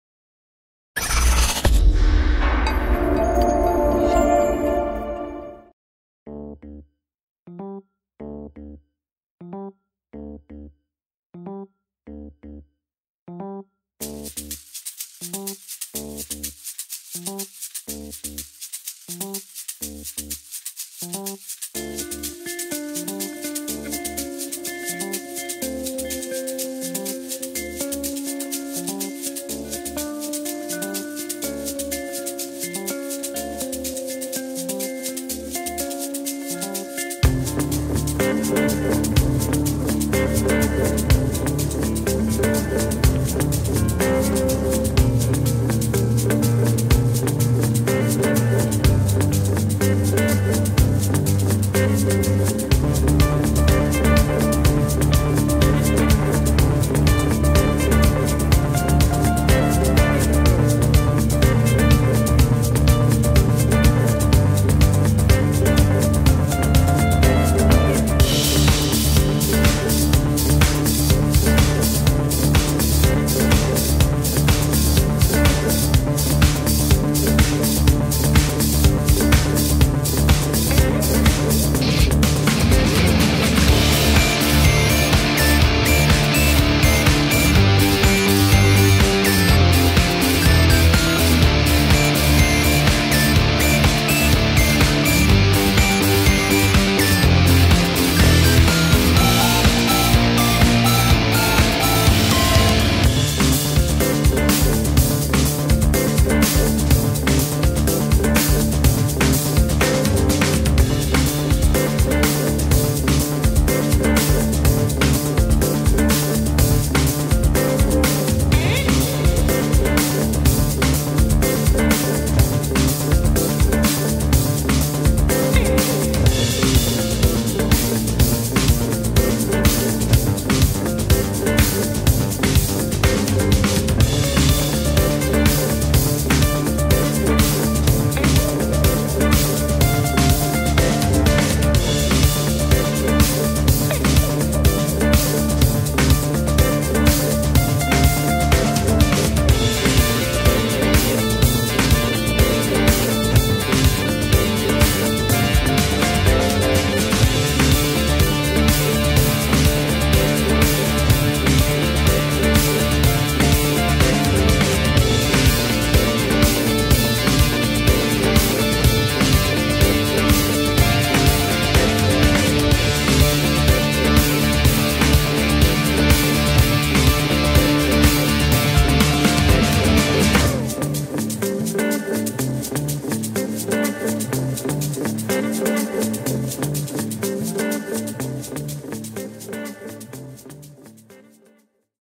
Evolights iQ-132B – Beam 2R Moving Head Głowa Ruchoma 132W
Evolights iQ-132B to wysokiej klasy ruchoma głowica estradowa typu beam, przeznaczona do profesjonalnych zastosowań. Reflektor wyposażony został w mocną lampę wyładowczą Osram Sirius HRI 132W, posiada statyczną tarczę gobo z efektem Gobo Shake, na której mieści się 17 nowoczesnych wzorów, a także tarczę kolorów, na której znajduje się 14 filtrów barwiących światło. Rozbicie wiązki umożliwia 8-ramienna pryzma. Wśród systemów zainstalowanych w głowicy warto na pewno wymienić zmotoryzowany focus, szybki shutter, funkcję autorepozycji, a także tryb stroboskopu z regulacją częstotliwości błysków.